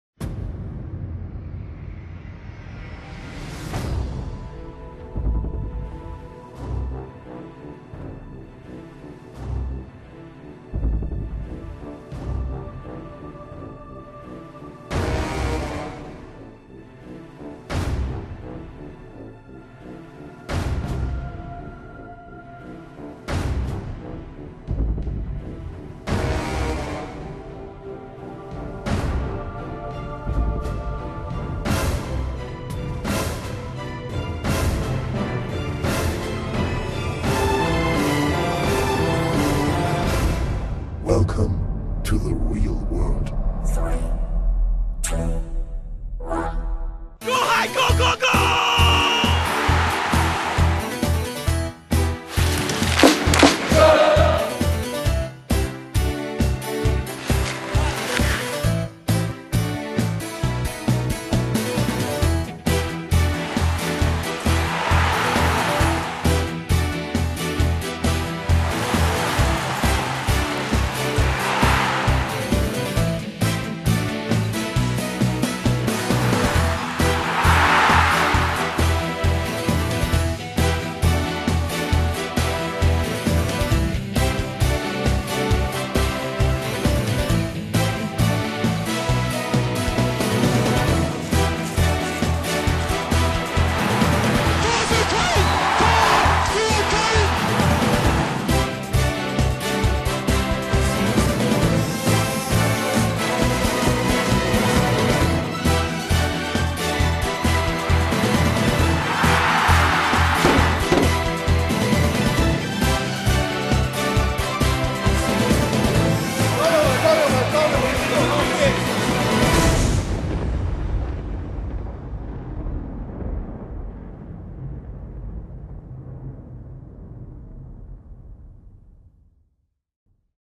Intro der Kölner Haie | Saison 2006/2007
Unser Intro aus der Saison 2006/2007
Mit Szenen aus der Haie-Kabine, aus Köln, mit Fans und auf dem Eis.